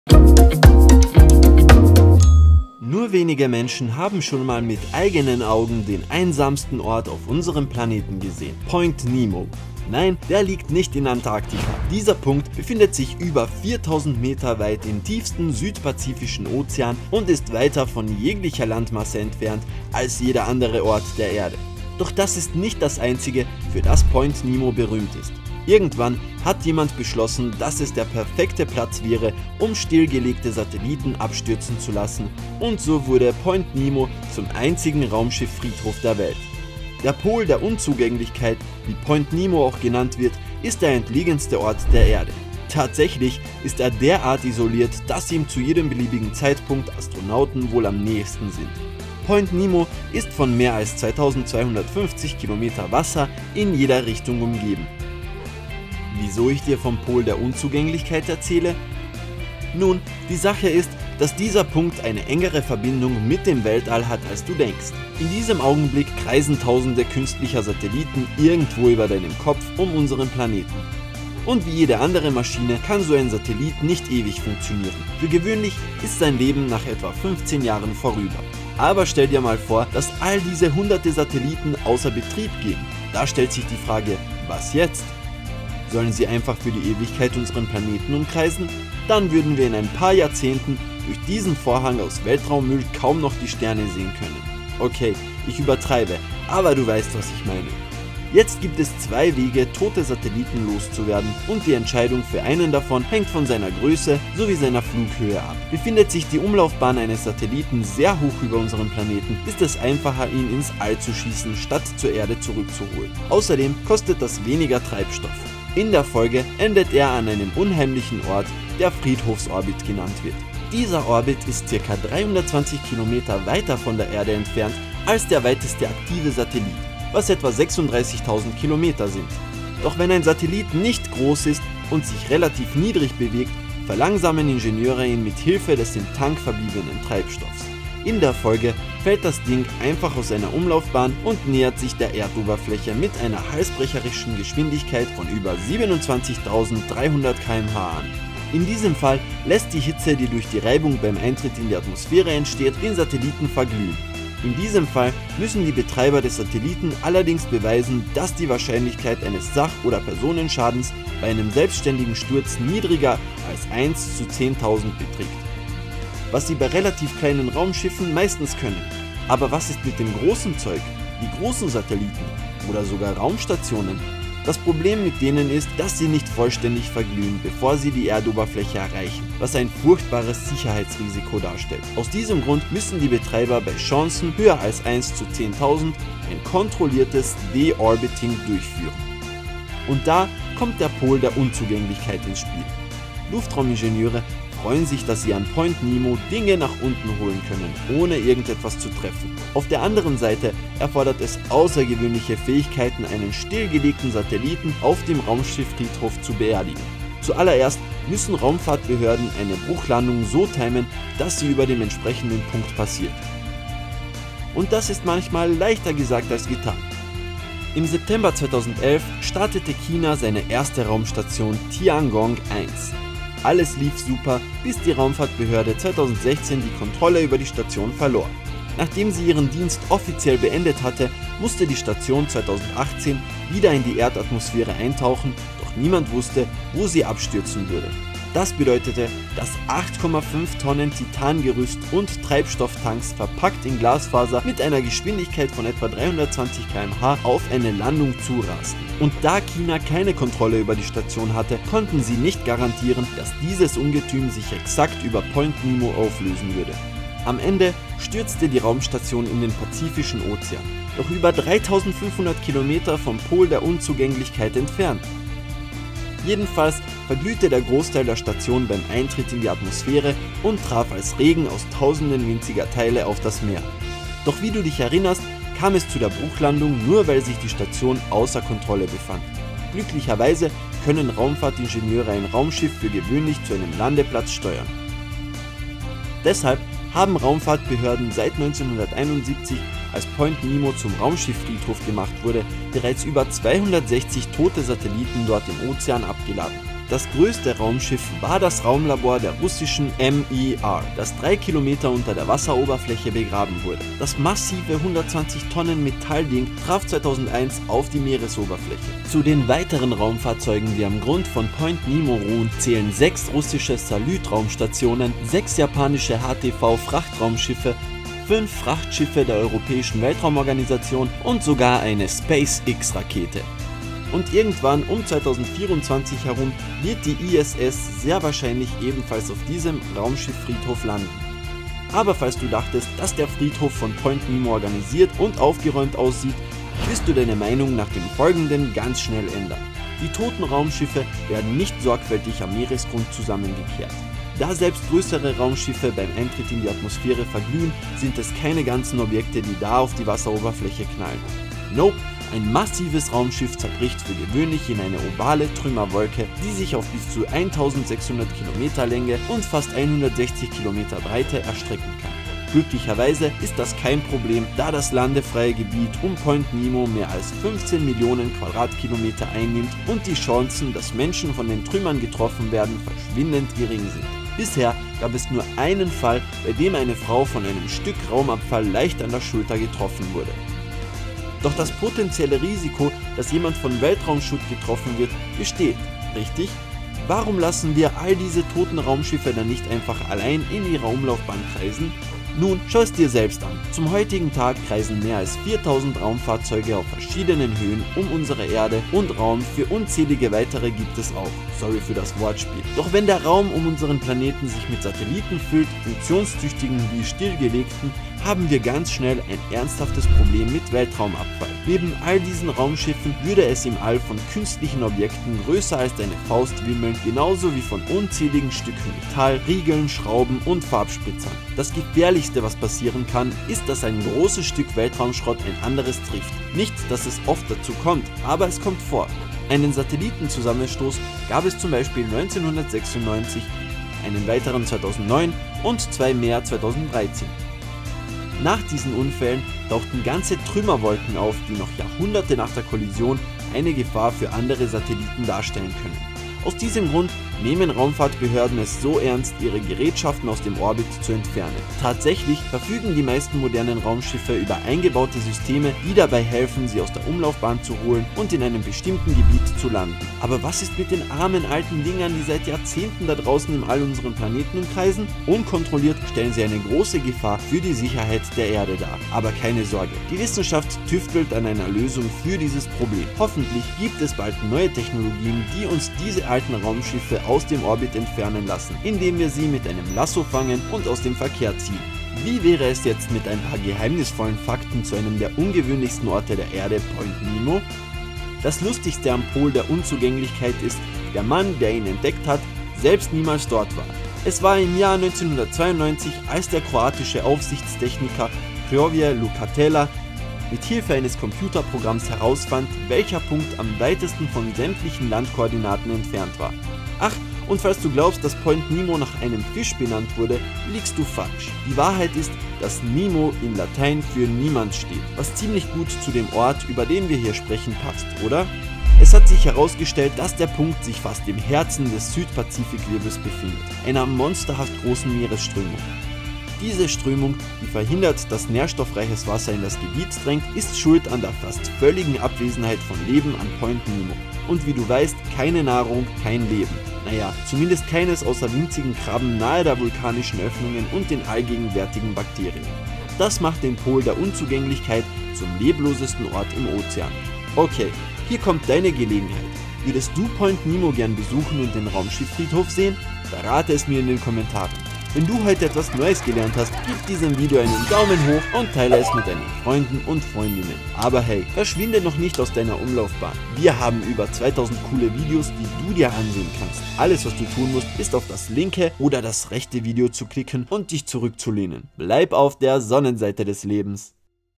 Am entlegensten Punkt der Erde hat man einen Raumschifffriedhof gefunden
Nur wenige Menschen haben schon mal mit eigenen Augen den einsamsten Ort auf unserem Planeten gesehen, Point Nemo. Nein, der liegt nicht in Antarktika.  Dieser Punkt befindet sich über 4.000 m weit im tiefsten Südpazifischen Ozean. Doch das ist nicht das Einzige, für das Point Nemo berühmt ist. Irgendwann hat jemand beschlossen, dass es der perfekte Platz wäre, um stillgelegte Satelliten abstürzen zu lassen - und so wurde Point Nemo zum einzigen Raumschifffriedhof der Welt.
Der Pol der Unzugänglichkeit, wie Point Nemo auch genannt wird, ist der entlegenste Ort der Erde. Tatsächlich ist er derart isoliert, dass ihm zu jedem beliebigen Zeitpunkt Astronauten wohl am nächsten sind! Die Sache ist, dass dieser Punkt eine engere Verbindung mit dem Weltall hat als du denkst!
TIMESTAMPS:
Der Meerespol der Unzugänglichkeit 0:33
Zwei Methoden, um tote Satelliten loszuwerden 1:37
Wie man einen stillgelegten Satelliten “beerdigt” 3:10
Wie viele tote Satelliten es gibt 4:30
Wie der Raumschifffriedhof aussieht 5:27
Das Problem mit dem Raumabfall 6:16
Ein paar beeindruckende Fakten über Point Nemo 7:55

SUMMARY:
- Es gibt zwei Wege, tote Satelliten loszuwerden. Befindet sich die Umlaufbahn eines Satelliten sehr hoch über unserem Planeten, ist es einfacher ihn ins All zu schießen statt zur Erde zurückzuholen.
- Doch wenn ein Satellit nicht groß ist und sich relativ niedrig bewegt, verlangsamen Ingenieure ihn mithilfe des im Tank verbliebenen Treibstoffs. In der Folge fällt das Ding einfach aus seiner Umlaufbahn und nähert sich der Erdoberfläche mit einer halsbrecherischen Geschwindigkeit von über 27.300 km/h an.
- Was ist mit dem großen Zeug, wie großen Satelliten oder sogar Raumstationen? Das Problem mit denen ist, dass sie nicht vollständig verglühen, bevor sie die Erdoberfläche erreichen, was ein furchtbares Sicherheitsrisiko darstellt.
- Da kommt der Pol der Unzugänglichkeit ins Spiel. Luftraumingenieure freuen sich, dass sie an Point Nemo "Dinge nach unten holen können" ohne irgendetwas zu treffen.
- Seit 1971, als Point Nemo zum Raumschifffriedhof gemacht wurde, haben Raumfahrtbehörden bereits über 260 tote Satelliten dort im Ozean abgeladen. Das größte Raumschiff war der Raumlabor der russischen MIR, das 3 km unter der Wasseroberfläche begraben wurde.
- Die toten Raumschiffe werden nicht sorgfältig am Meeresgrund zusammengekehrt. Da selbst größere Raumschiffe beim Eintritt in die Atmosphäre verglühen, sind es keine ganzen Objekte, die da auf die Wasseroberfläche knallen.
- Wenn der Raum um unseren Planeten sich mit Satelliten füllt, funktionstüchtigen wie stillgelegten, haben wir ganz schnell ein ernsthaftes Problem mit Weltraumabfall.
- Das Gefährlichste, was passieren kann, ist, dass ein großes Stück Weltraumschrott ein anderes trifft. Nicht, dass es oft dazu kommt, aber es kommt vor.
- Die meisten modernen Raumschiffe verfügen über eingebaute Systeme, die dabei helfen, sie aus der Umlaufbahn zu holen und in einem bestimmten Gebiet zu landen.
- Das Lustigste am Pol der Unzugänglichkeit ist, der Mann, der ihn entdeckt hat, selbst niemals dort war! Es war im Jahr 1992, als der kroatische Aufsichtstechniker Hrvoje Lukatela mithilfe eines Computerprogramms herausfand, welcher Punkt am weitesten von sämtlichen Landkoordinaten entfernt war.